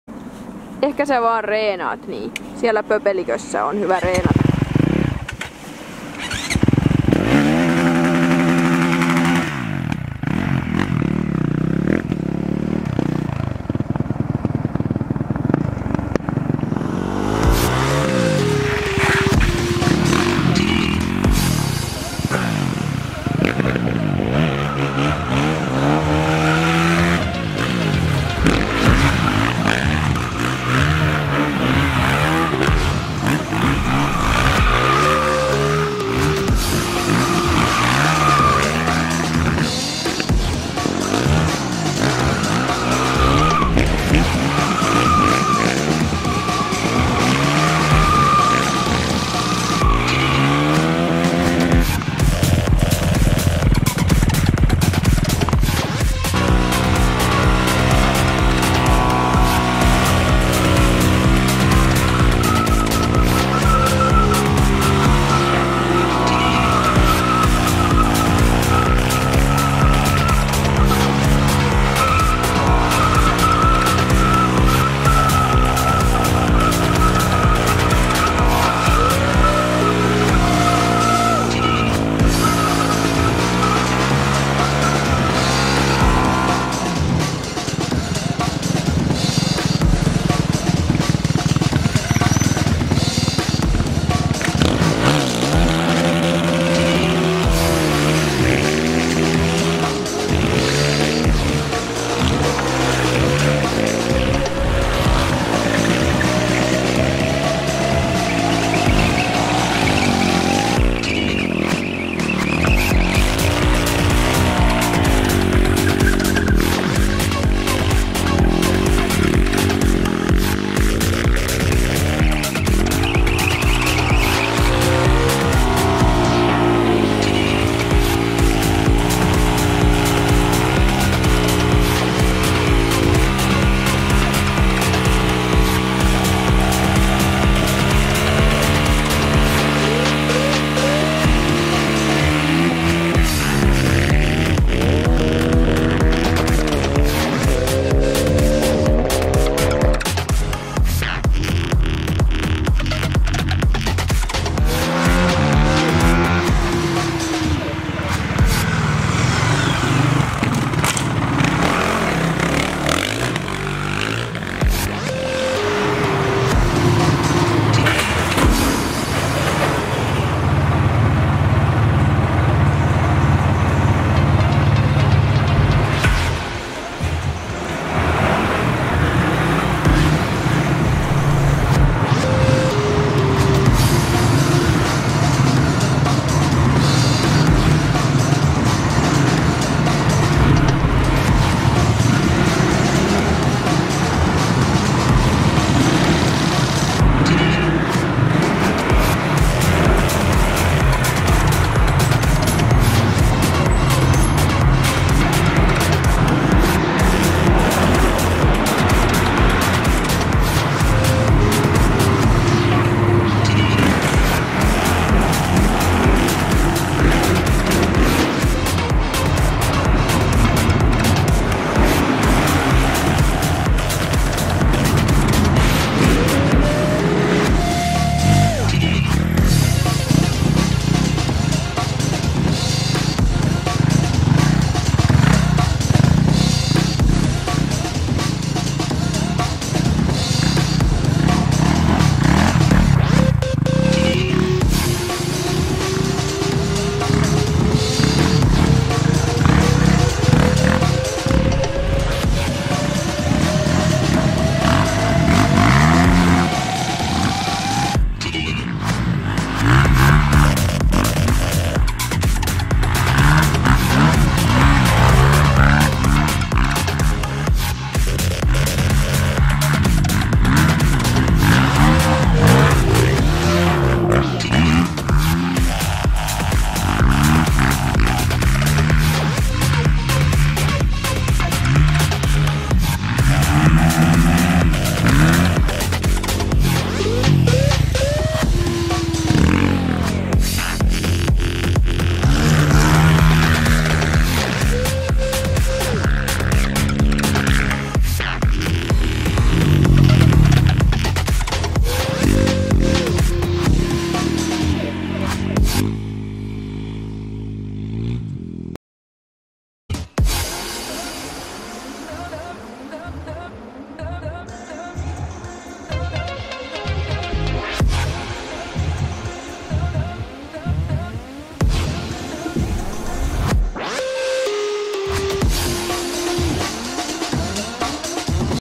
Arttu Stenberg | Best Of 2019 Instagram
Best of my Instagram clips with Honda CRF 450L
- Season 2019